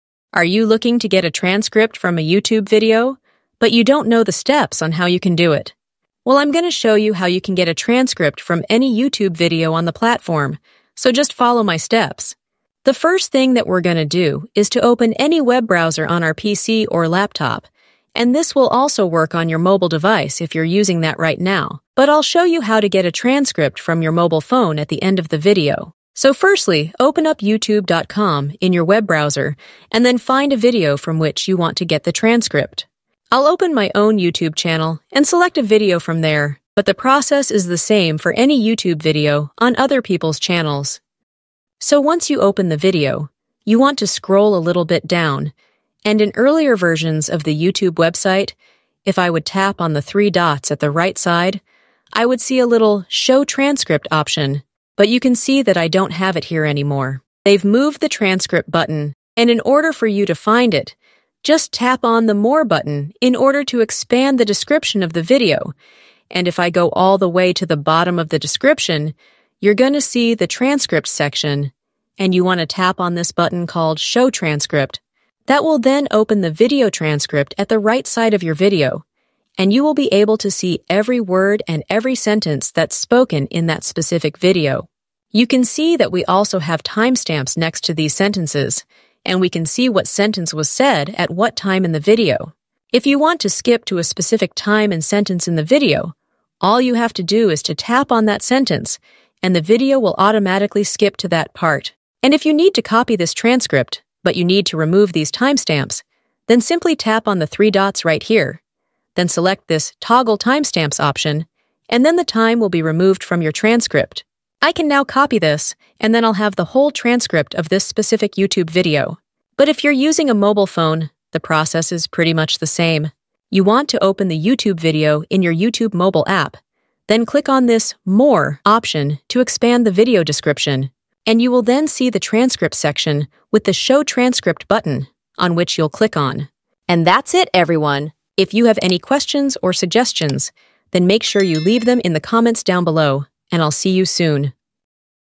How To Get Transcript From YouTube Video (2024)
Want to extract a transcript from a YouTube video in 2024? This video will show you how! Whether you're looking to save time, take notes, or use the transcript for research, this step-by-step guide will make the process simple and quick.